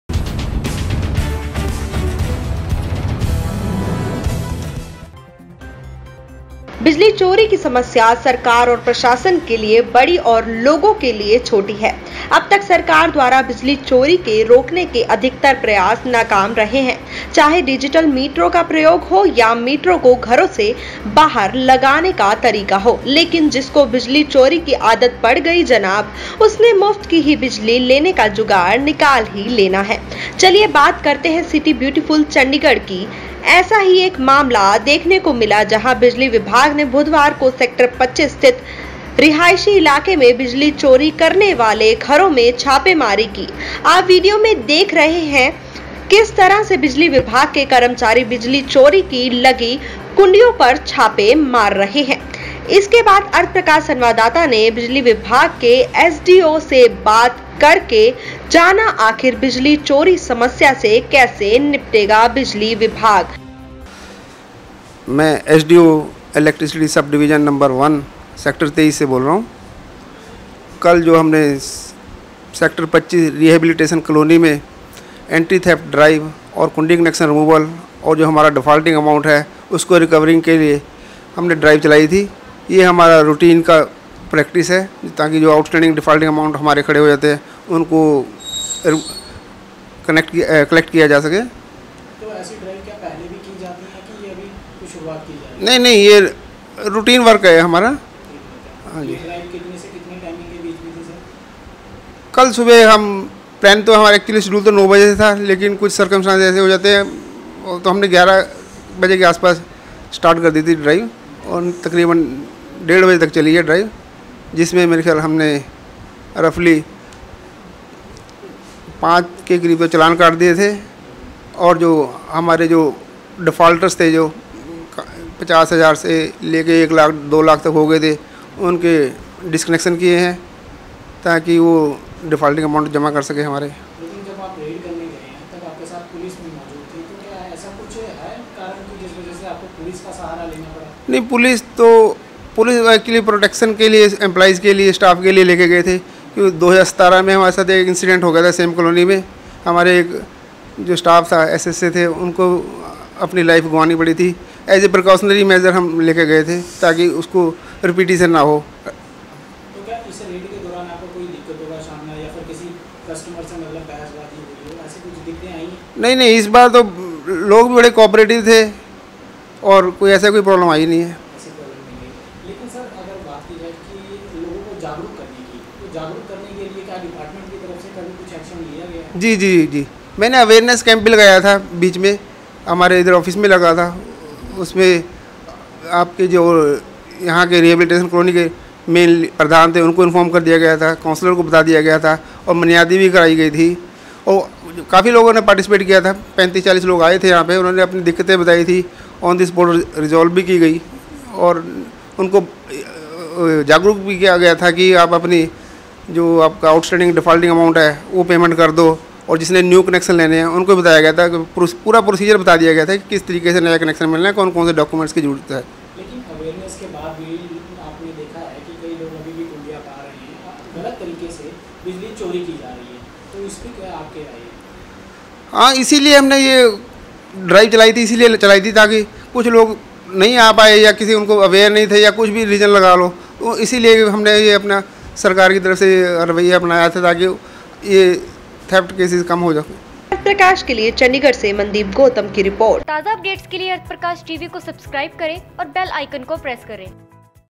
WATCH चंडीगढ़ में बिजली चोरी करने वालों पर चला विभाग का डंडा
बिजली चोरी की समस्या सरकार और प्रशासन के लिए बड़ी और लोगों के लिए छोटी है, अब तक सरकार द्वारा बिजली चोरी के रोकने के अधिकतर प्रयास नाकाम रहे हैं चाहे डिजिटल मीटरों का प्रयोग हो या मीटरों को घरों से बहार लगाने का तरीका हो, लेकिन जिसको बिजली चोरी की आदत पड़ गयी जनाब उसने मुफ्त की बिजली लेने का जुगाड़ निकाल ही लेना है चलिए बात करते हैं सिटी ब्यूटीफुल चंडीगढ़ की ऐसा ही एक मामला देखने को मिला जहाँ  बिजली विभाग ने बुधवार को सेक्टर 25 स्थित रिहायशी इलाके में बिजली चोरी करने वाले घरों में छापेमारी की आप वीडियो में देख रहे हैं किस तरह से बिजली विभाग के कर्मचारी बिजली चोरी की लगी कुण्डियों पर छापे मार रहे हैं इसके बाद अर्थ प्रकाश संवाददाता ने बिजली विभाग के एस डी ओ से बात करके जाना आखिर बिजली चोरी समस्या से कैसे निपटेगा बिजली विभाग